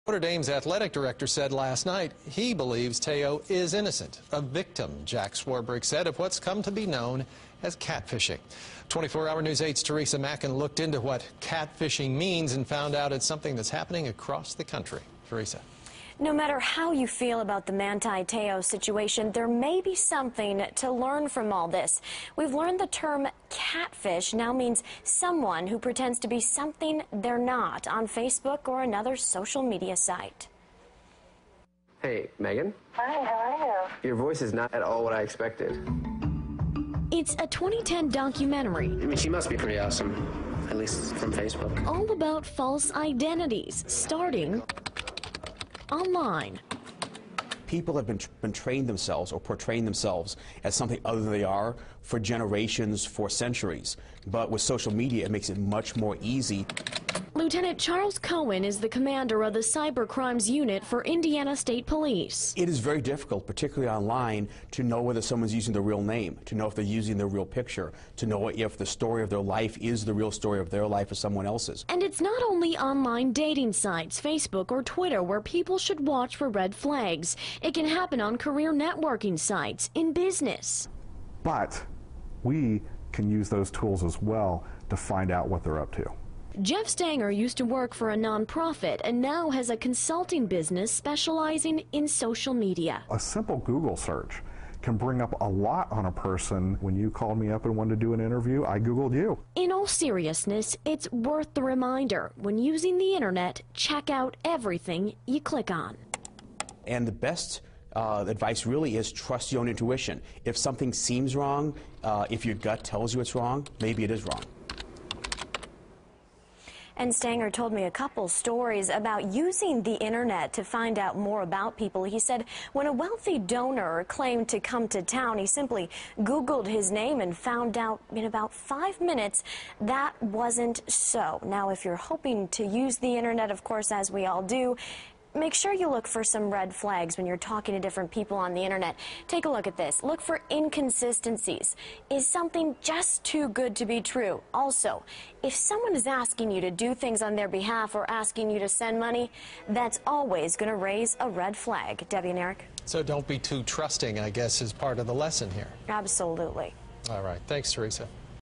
INDIANAPOLIS (WISH) - Wednesday night, Notre Dame'sathletic director said he believes Manti Te'o
What is "catfishing?"

Updated: Thursday, 17 Jan 2013, 10:40 PM EST
Published : Thursday, 17 Jan 2013, 10:29 PM EST

    Teresa Mackin

INDIANAPOLIS (WISH) - Wednesday night, Notre Dame's athletic director said he believes Manti Te'o is innocent. A victim, he said, of what's come to be known as "catfishing."

24-Hour News 8's Teresa Mackin looked into the term and found out -- it's happening across the country.

The term comes from the 2010 documentary, all about false identities starting online. "Catfish" is someone who pretends to be someone they're not, using Facebook or other social media to create false identities.

Lt. Charles Cohen investigates cyber crime with Indiana State Police.

He didn't comment on the situation at Notre Dame, but explained the dangers that can lurk on the internet.

"People have been portraying themselves as something other than they are, for generations," he explained. "But with social media, it makes it much more easy for me to befriend someone, that lives in a different geographic place. It makes it much more difficult for me to determine what the truth really is in some cases."

"It is very difficult, particularly online, to know if someone's using a real name, to know if they're using a real picture, or to know if story of their life is the real story of their life or someone else's," Lt. Cohen added.

It's not only Facebook, Twitter, or online dating sites where people should watch for red flags.

It's everywhere on the internet -- whether it's a career networking site, or a site like Craigslist.

Examples of red flags on the Internet:

    Inconsistencies
    Ask you to do things on their behalf -- send money somewhere or pick up packages for them